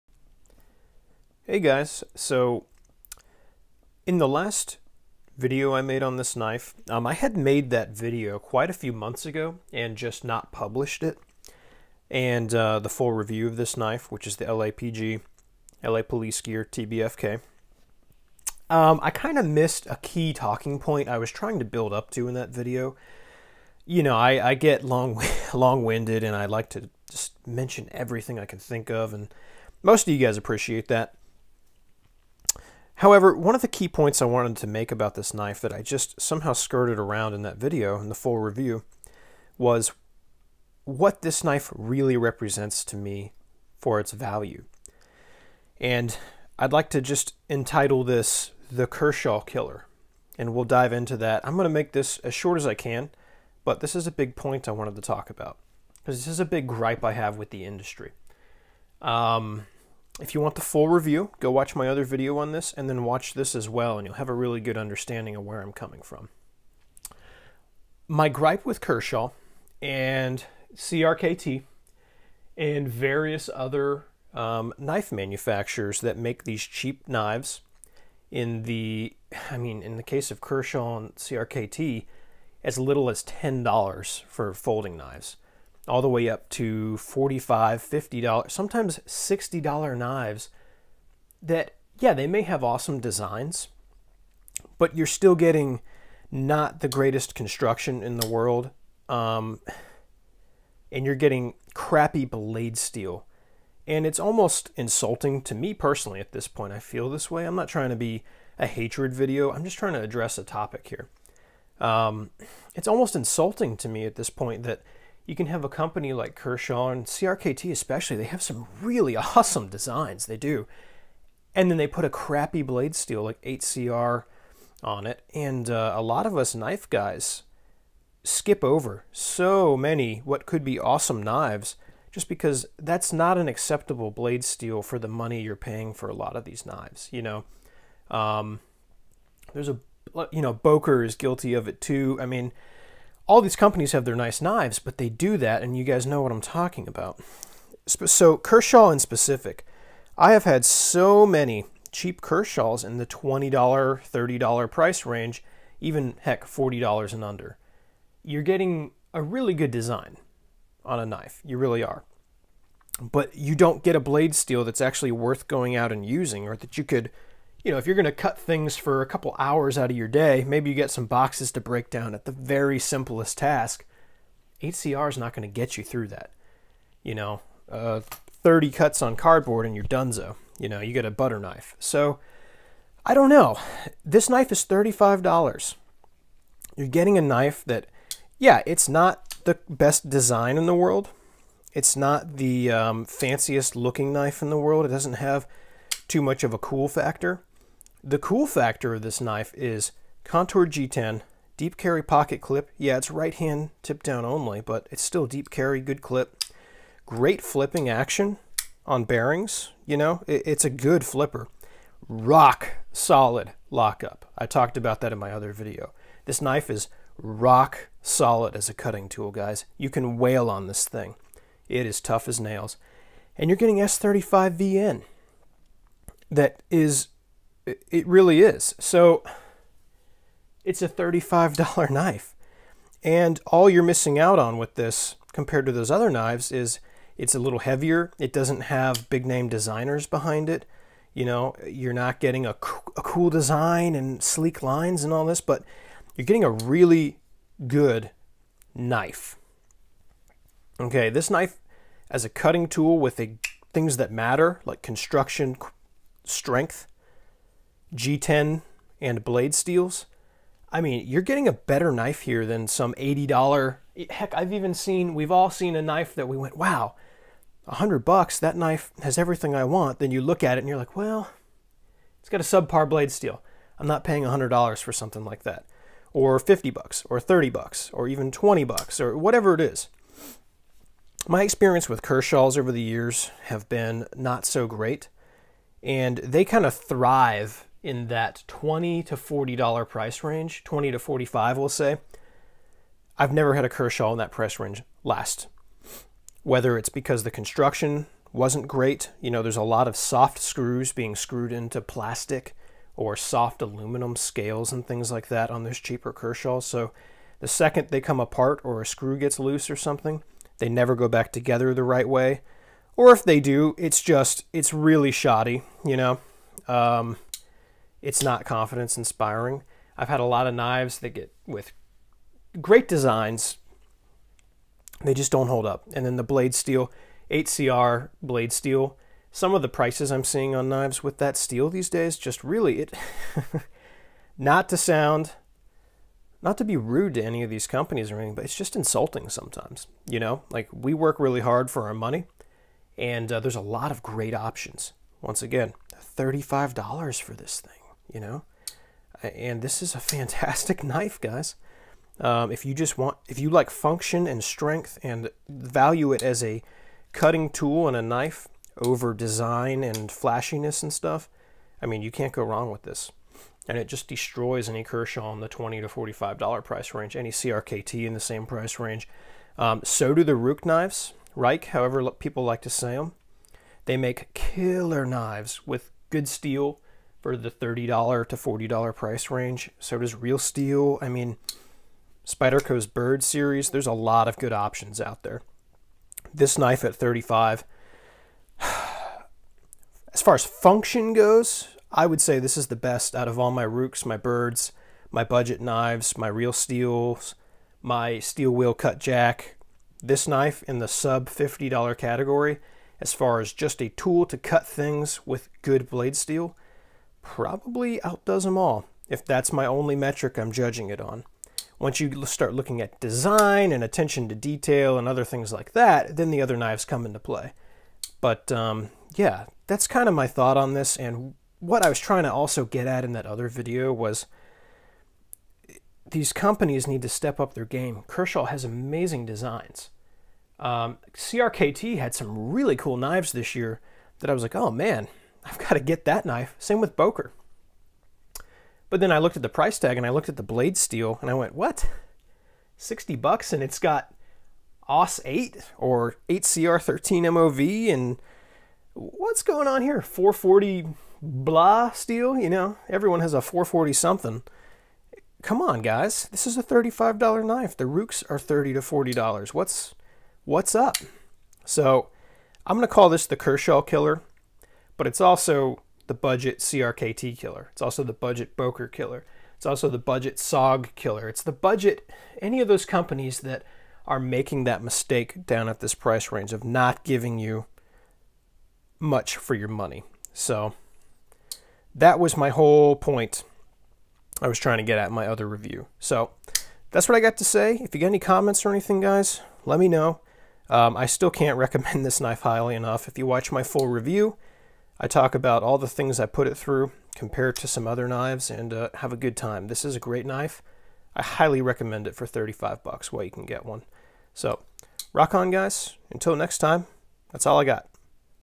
Value In The Knife World. What is it?
Please Like and Thumbs up!  It REALLY Helps my exposure!
-Expanding on some further thoughts on this knife and the value it represents in the industry and some aggravation with what is considered a "value". Rock on!